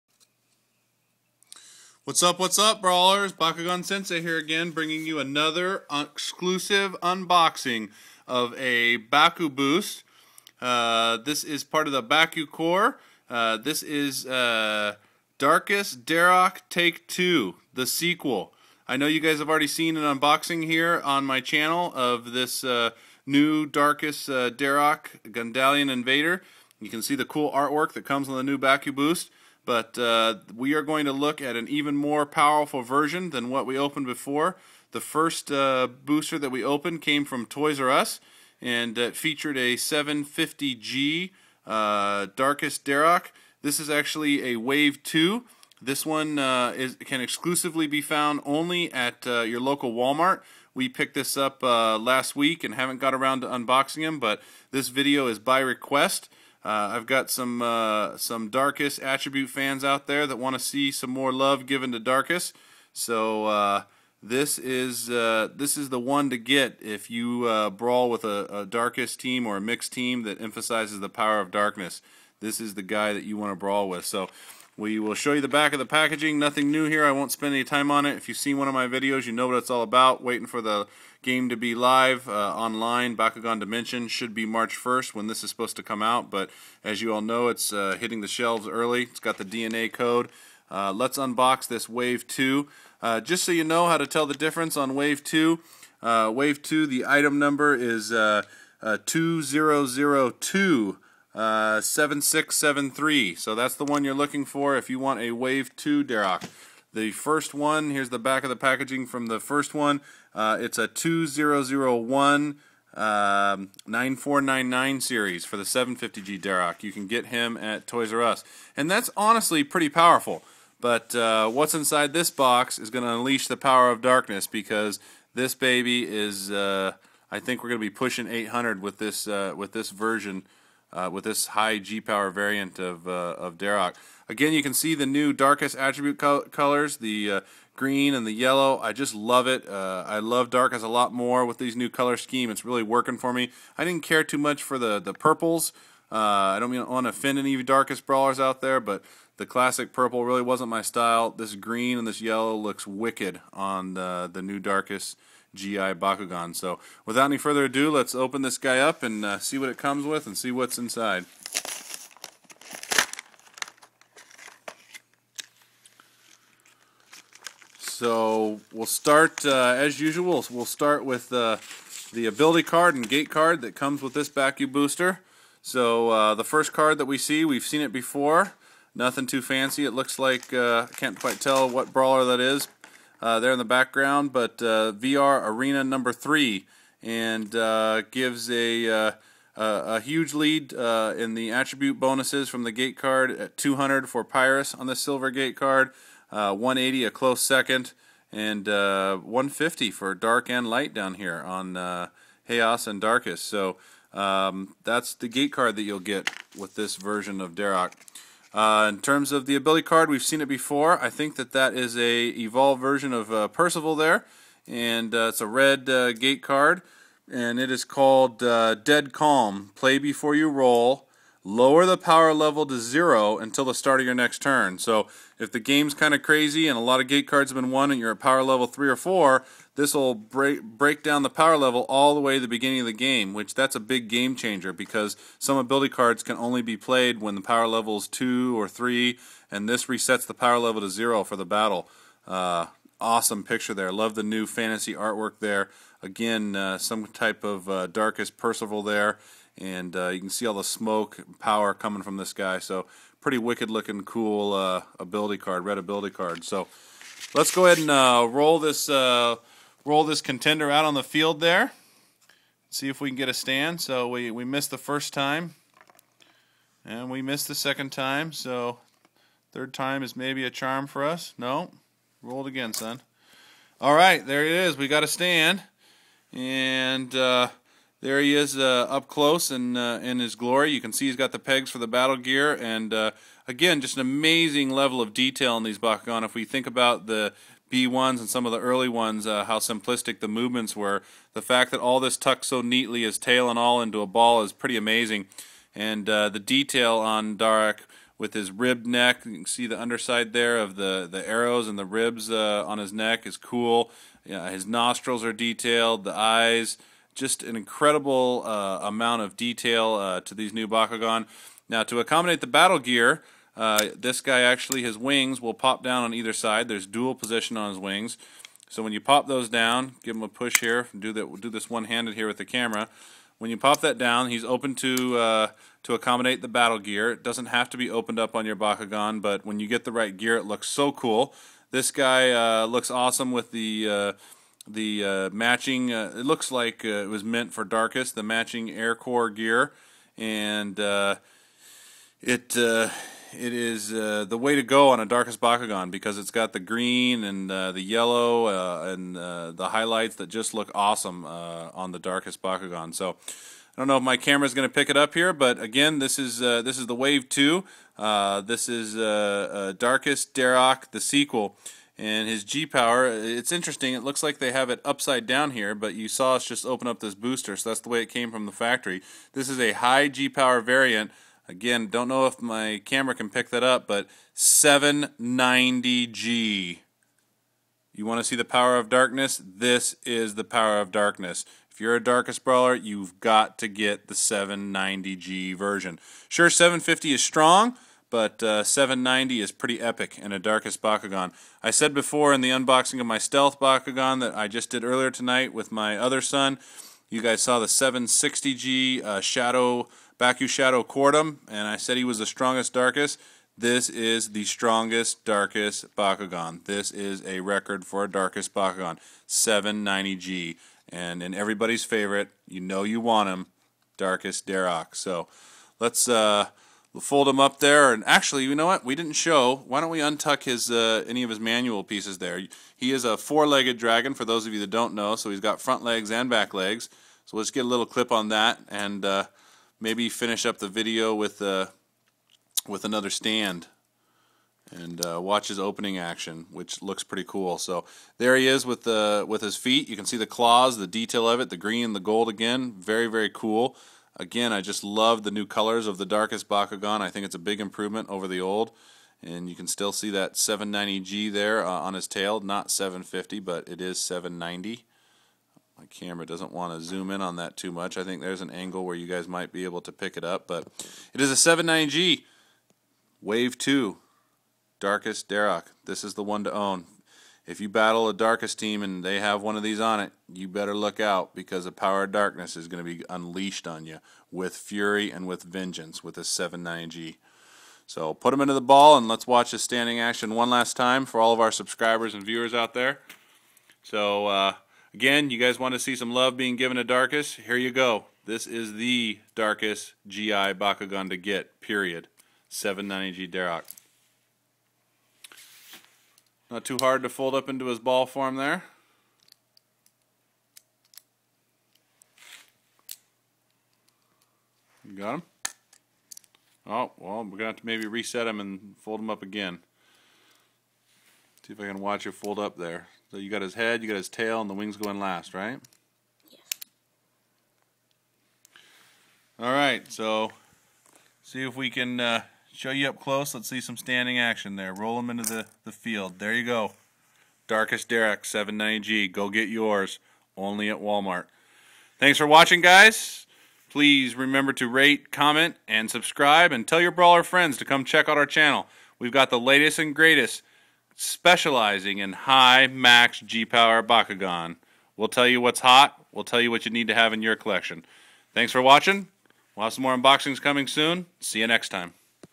Bakugan Gundalian Invaders Darkus Dharak WAVE 2 790g unboxing
BakuganSensei here with a SPECIAL TREAT by request from my Darkus brawler subscribers and fans. You think Darkus isn't getting any love ? In this episode, my 8 year old son tears into the newest addition to his Bakugan lineup.  This WAVE 2 high g-power variant Darkus Dharak  will ensure you a victorious outcome in your brawls ! Few opponents can whether the storm unleashed by this mighty beast ! Darkus to Victory !  Let's Brawl !